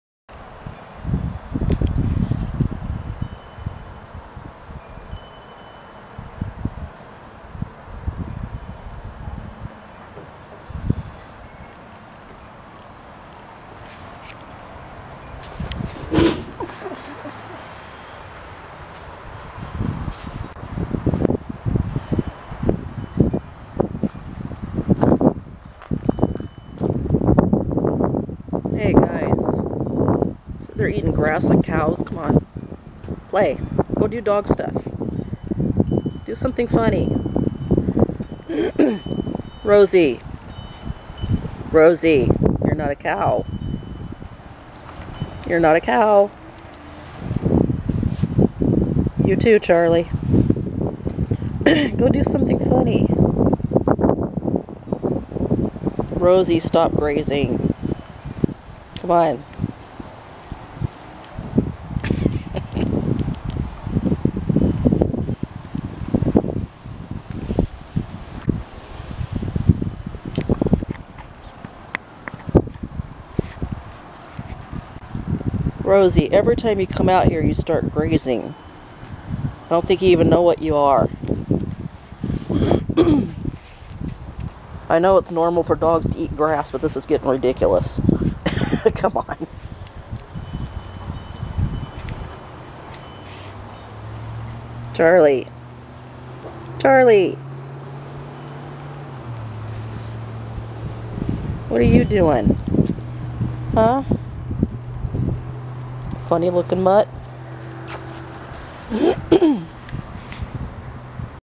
Species-Confused Dog, Indifferent Birds: Backyard Adventures
Half-tame sparrows look on at Rosie the (cow?) dog, who has lately developed quite a fondness for grazing. She has even gotten Charlie the dog finding his inner vegan as well.